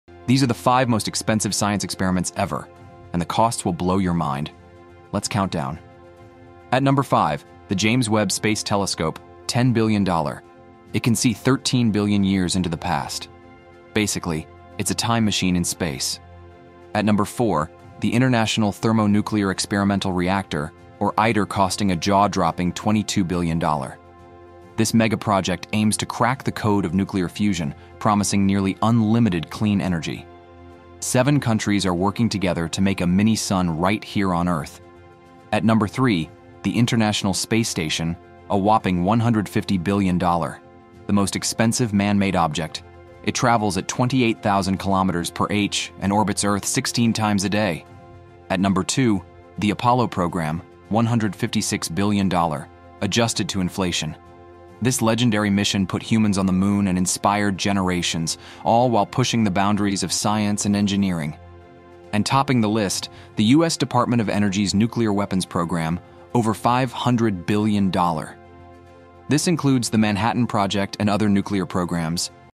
5 most expensive science experiments ever
Discover the 5 most expensive scientific experiments in history, from space telescopes to nuclear secrets! These mind-blowing projects pushed the boundaries of science—and budgets! Perfect for science lovers and curious minds.